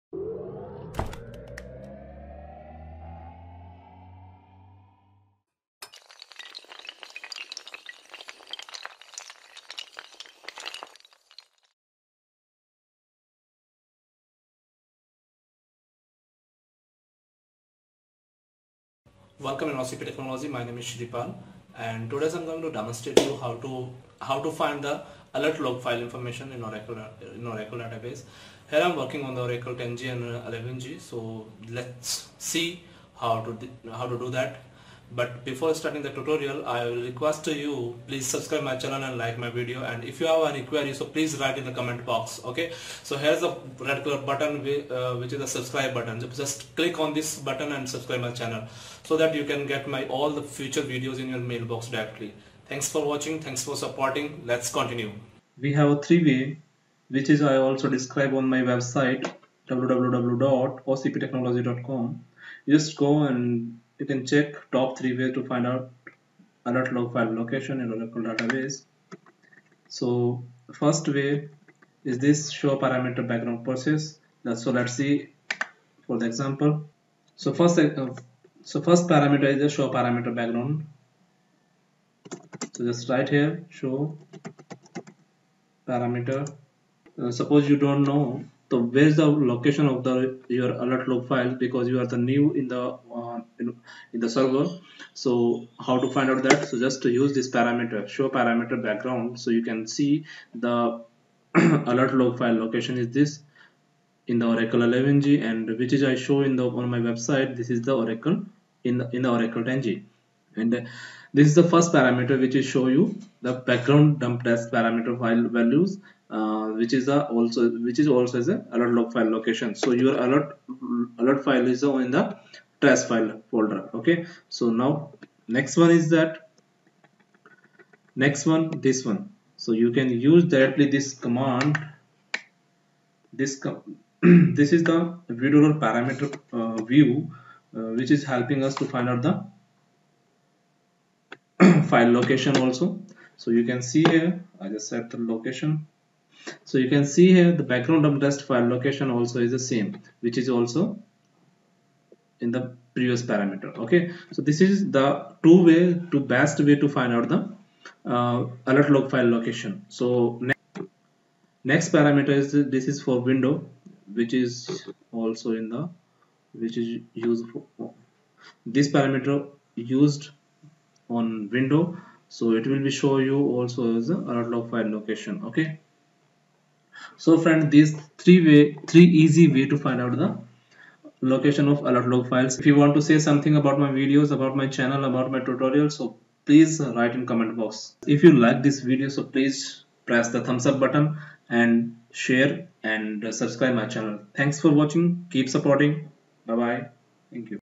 Top 3 way | How to find Alert log file location in ORACLE 10g | ORACLE 11g
Hello friends in this video i'm going to show you how to find alert log file location if you are new on server, this video is useful to all of you, if you like my video so please subscribe my channel and like & share my video.